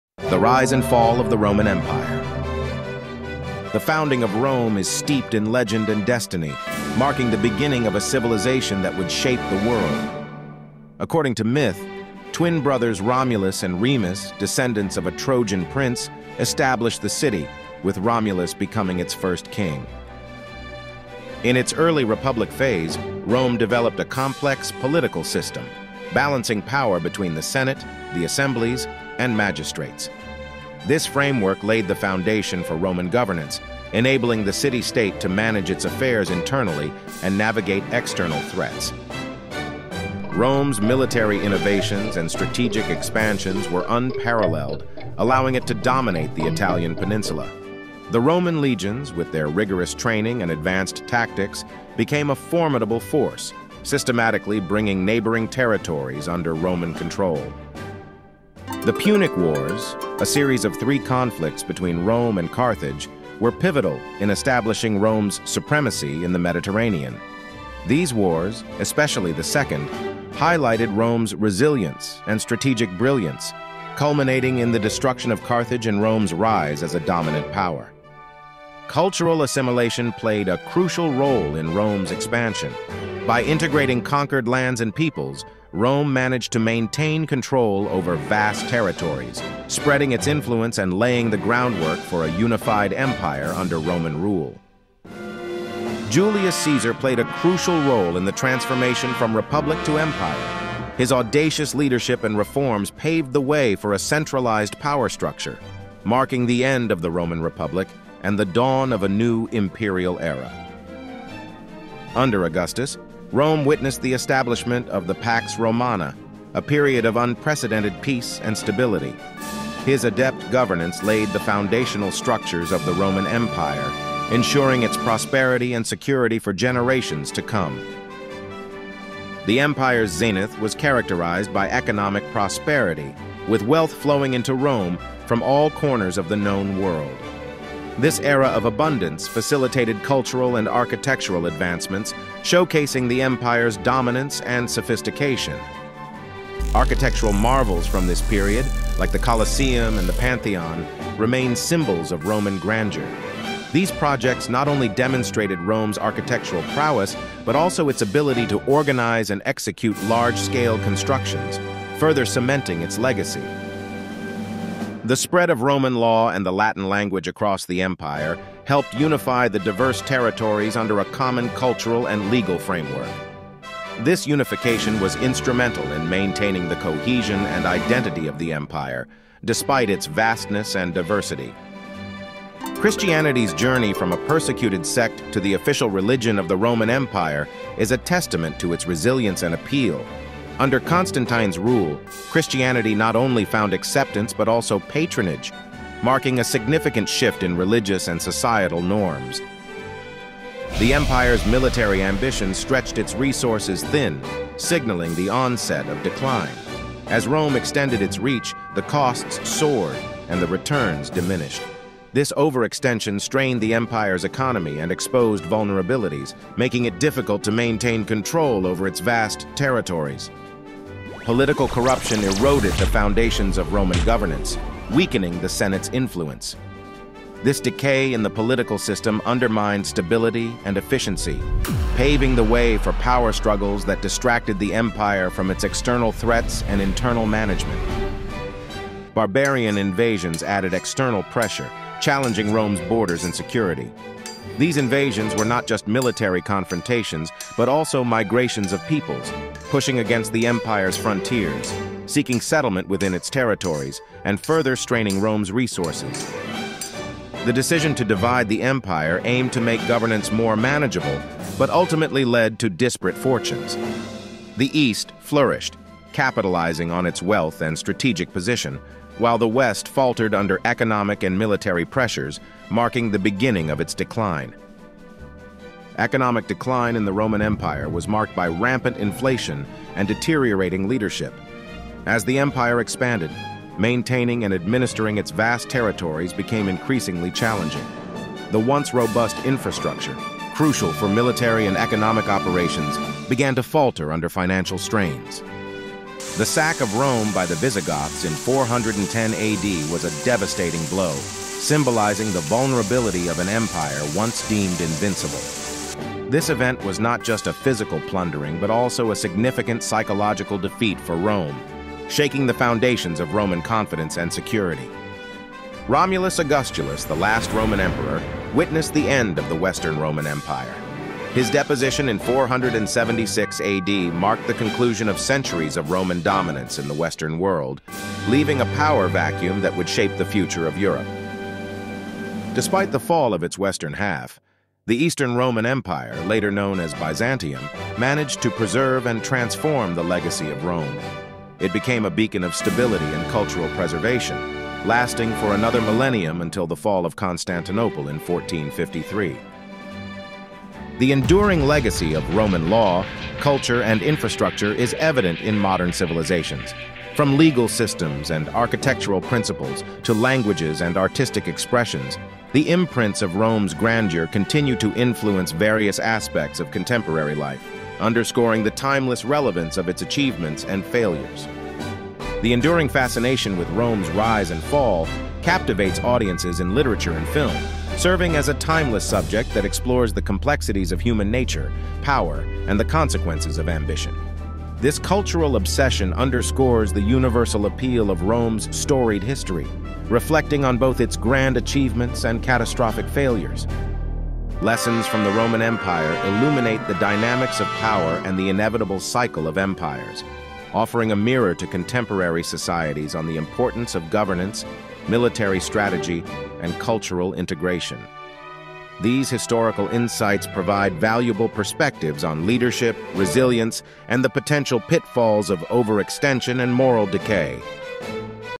Exploring the Roman Empire Craze, Unraveling History's Mysteries
Dive into the heart of history with our latest video exploring the timeless fascination with the Roman Empire, now trending on TikTok with over 1.6 billion views! Discover why "my Roman Empire" has captivated millions and how ancient themes resonate in modern times. Plus, journey through the African continent's rich past, as recent genetic studies unveil the profound impact of ancient African empires on global migration patterns. Don't miss out on the chance to connect with the past through these compelling narratives that continue to shape our understanding of history.